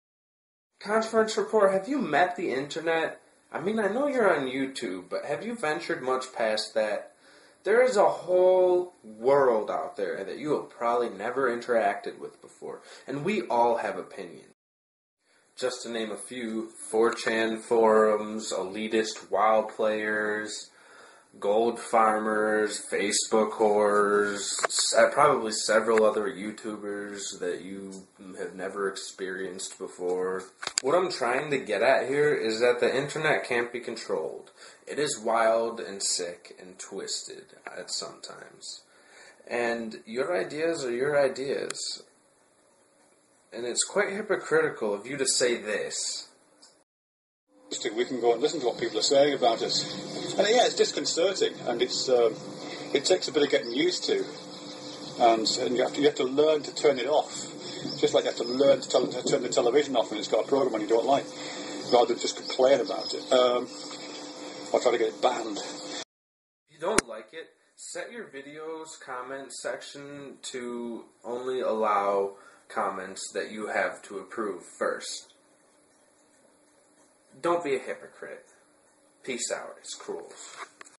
re: Rudeness on Youtube
If you don't apriciate the comments then just have them deleted. Don't make a video complaining.